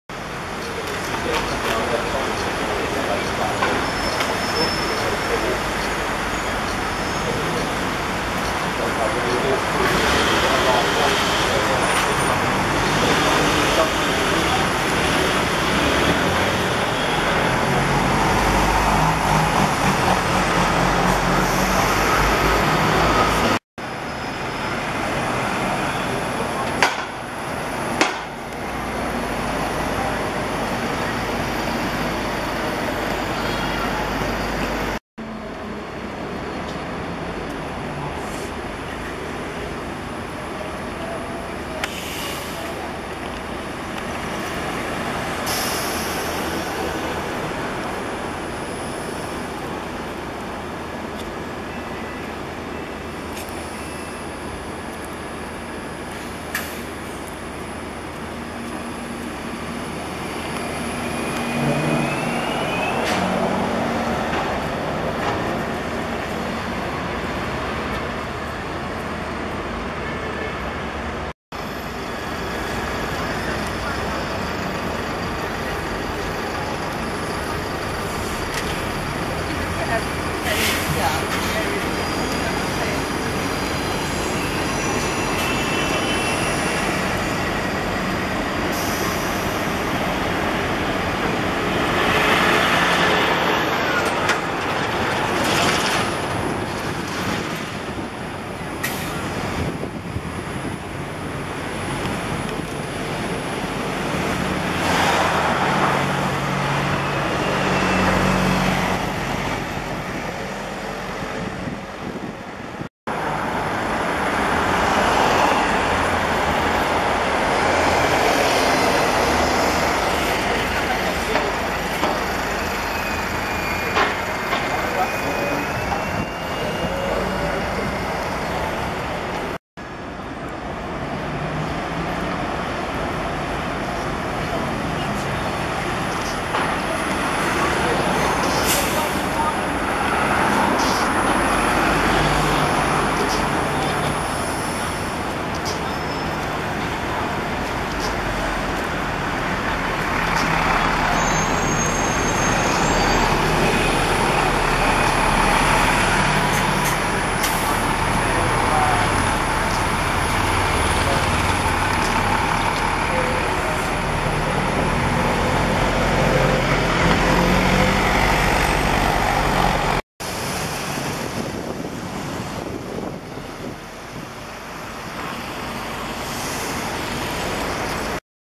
First Day of New Routemasters on London Bus Route 8- 28 June 2014
Some of the new Routemaster buses on London bus route 8 during the morning of Saturday 28 June 2014, which was their first day of operation on this route. The video was taken in the Liverpool Street area.